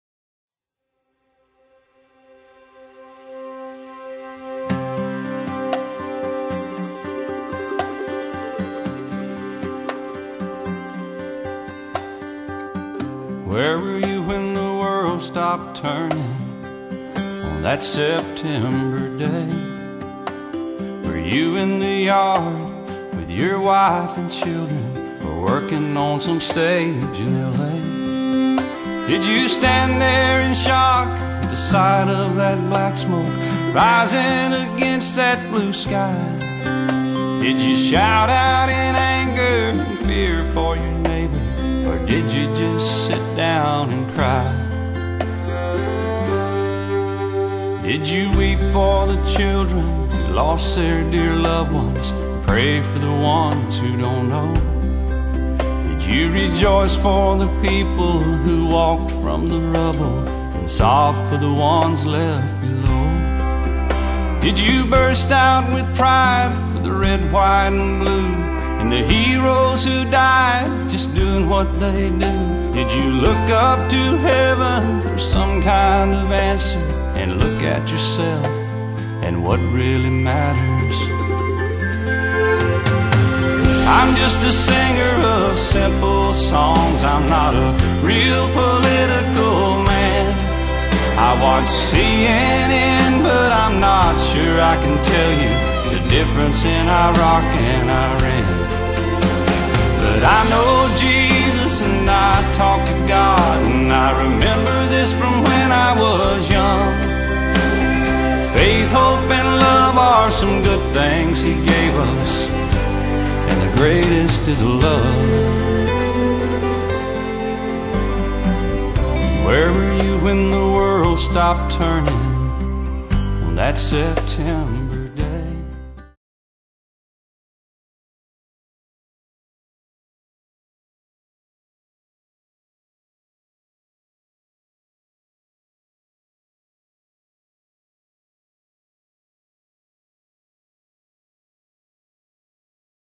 9/11 Memorial
Taken in Greenfield Indiana at Gander Outdoor!
Shot on Yuneec Typhoon H!